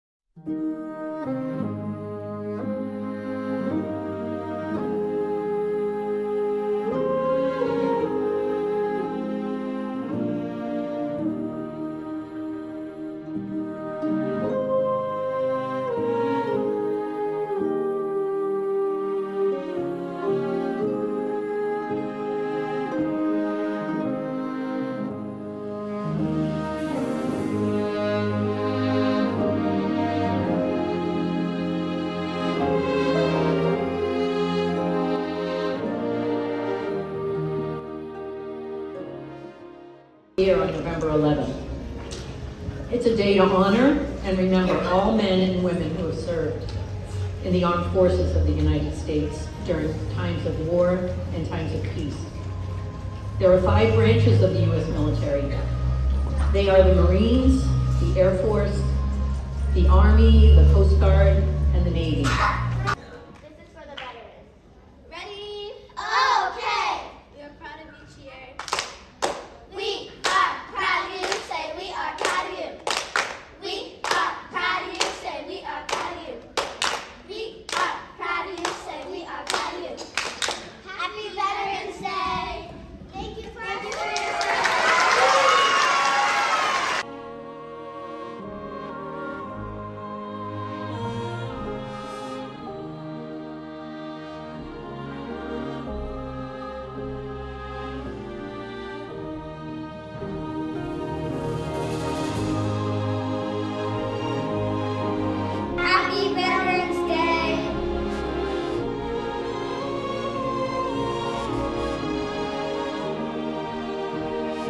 Happy Veterans Day From Waddell Elementary
Waddell elementary honoring our veterans.

Happy Veterans Day from Manchester Public Schools!

See What Our District Is Like!